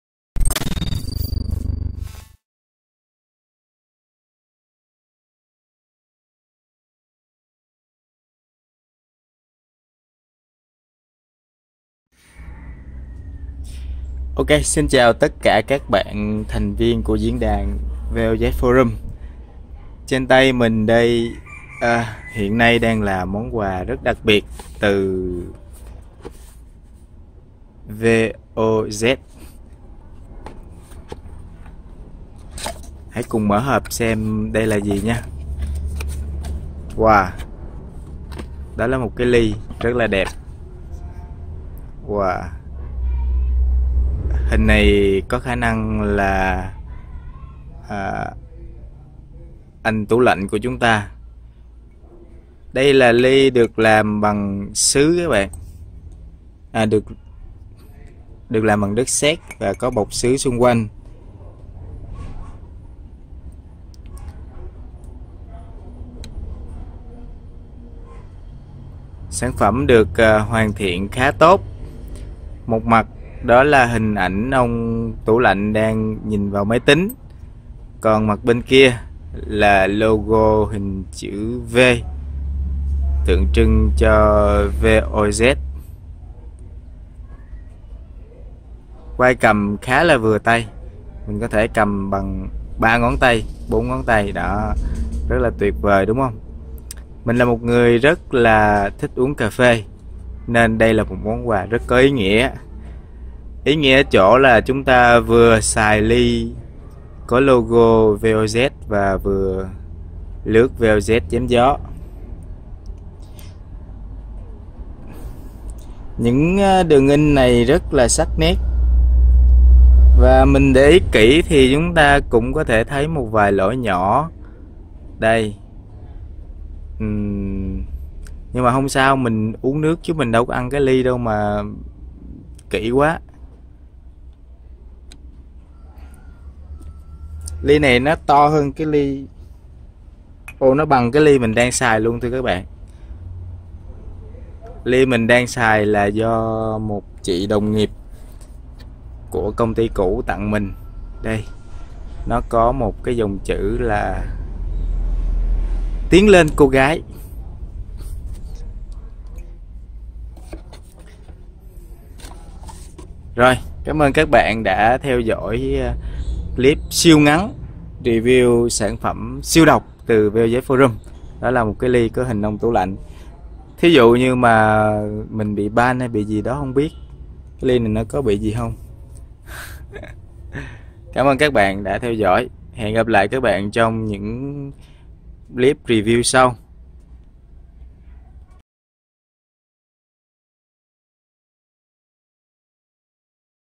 Mở hộp và trên tay quà tặng từ vozForums phiên bản đặc biệt giới hạn!
💥 Mở hộp và trên tay Ly uống nước (có thể uống cafe hay cái gì khác cũng được) phiên bản đặc biệt giới hạn của vozForums (forums.voz.vn)

👉 Like Facebook của Techs.Review để cập nhật nhiều thông tin mới
⭕️⭕️⭕️ Gear dùng để làm video này ⭕️